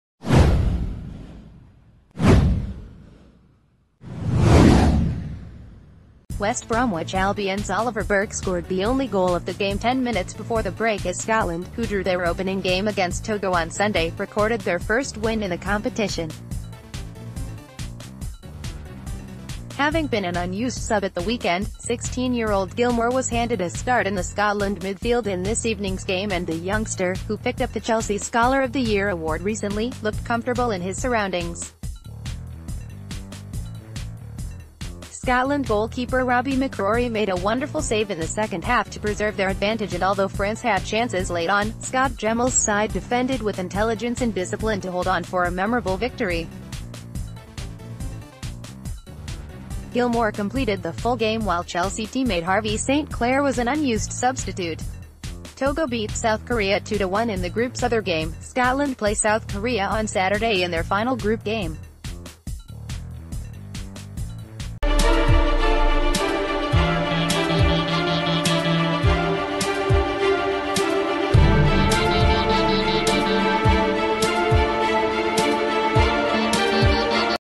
Internationals: Gilmour features in big win for Scots
West Bromwich Albion’s Oliver Burke scored the only goal of the game 10 minutes before the break as Scotland, who drew their opening game against Togo on Sunday, recorded their first win in the competition. Having been an unused sub at the weekend, 16-year-old Gilmour was handed a start in the Scotland midfield in this evening’s game and the youngster, who picked up the Chelsea Scholar of the Year award recently, looked comfortable in his surroundings. Scotland goalkeeper Robby McCrorie made a w...